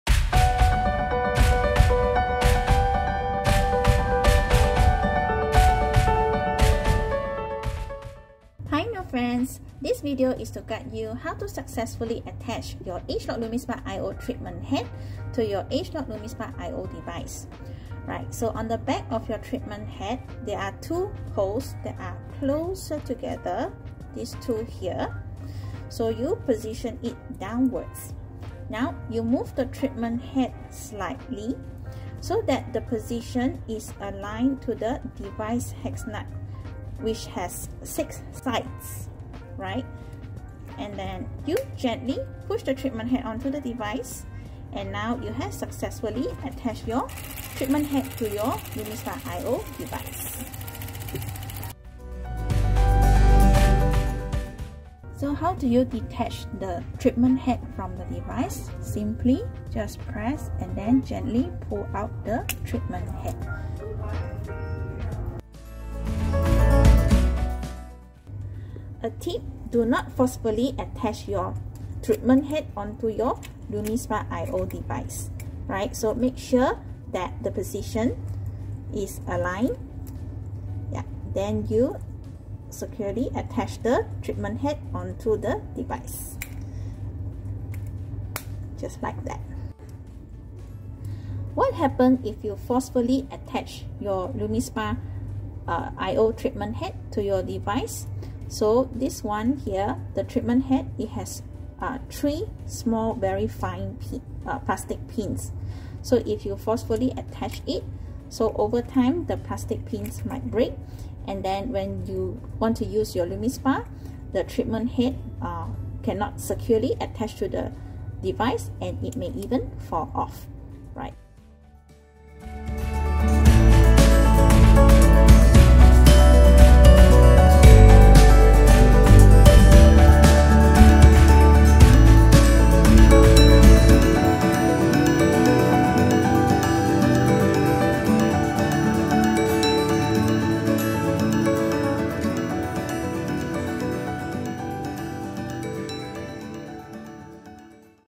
How To: ageLOC® LumiSpa® iO Treatment Head Guide | Nu Skin Pacific
Attach and detach your ageLOC® LumiSpa® iO Treatment Head the correct way. Our guide is here to show you how, see below for specific information.

00:06 - How to attach your ageLOC® LumiSpa® iO Treatment Head
00:54 - How to detach your ageLOC® LumiSpa® iO Treatment Head
01:11 - Bonus Tip!
01:36 - What happens if you forcefully attach your ageLOC® LumiSpa® iO Treatment Head